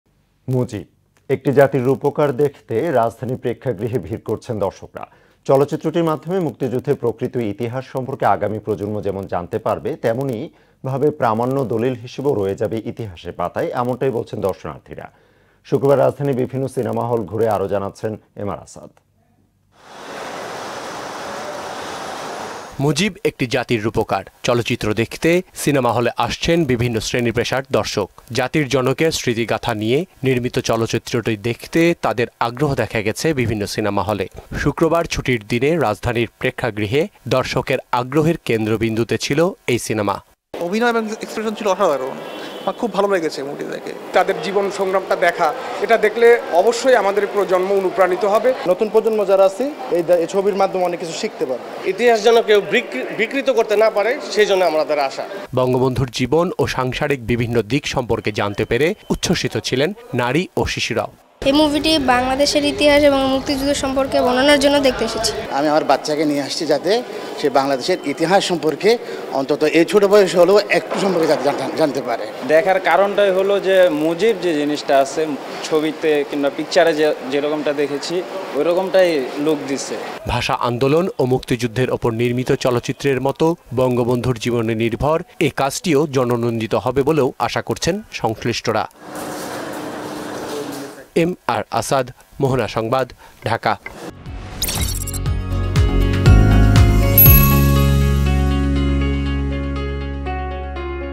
মুজিব একটি জাতির রূপকার দেখতে রাজধানীর প্রেক্ষাগৃহে ভিড় করছেন দর্শকরা | Mohona Tv
মুজিব-একটি জাতির রূপকার দেখতে রাজধানীর প্রেক্ষাগৃহে ভিড় করছেন দর্শকরা। চলচ্চিত্রটির মাধ্যমে মুক্তিযুদ্ধের প্রকৃত ইতিহাস সম্পর্কে আগামী প্রজন্ম যেমন জানতে পারবেন, তেমনি ভাবে প্রামাণ্য দলিল হিসেবেও রয়ে যাবে ইতিহাসের পাতায়, এমনটাই বলছেন দর্শনার্থীরা।